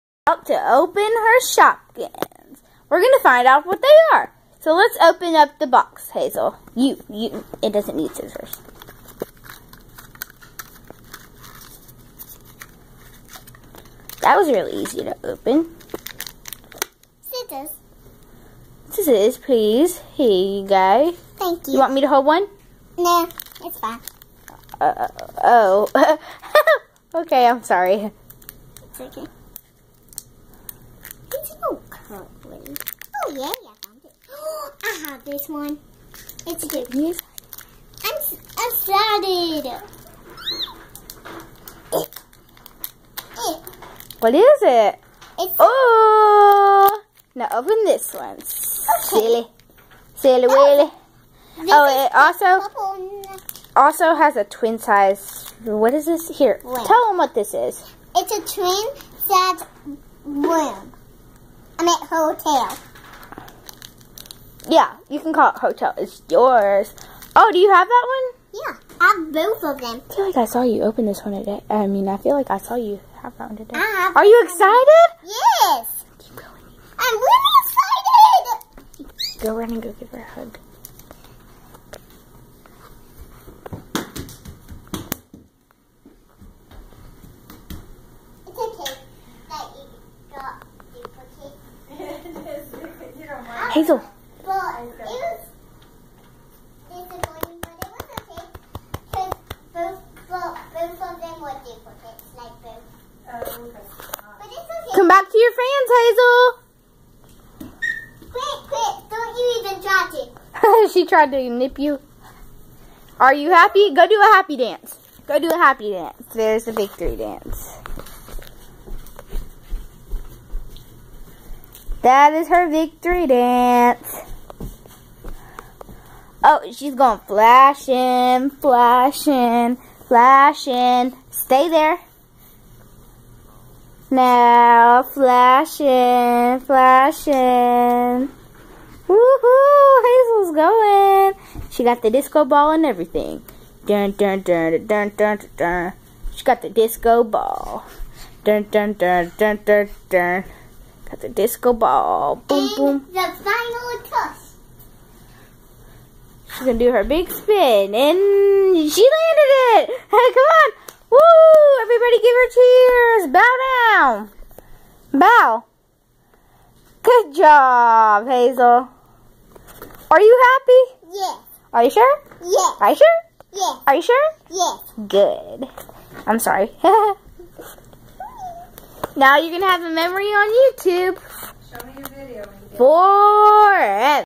Crazy for Shopkins
Hazel opening her Shopkins present. She just loves Shopkins, even when she discovers that her gift is a duplicate of ones she already has.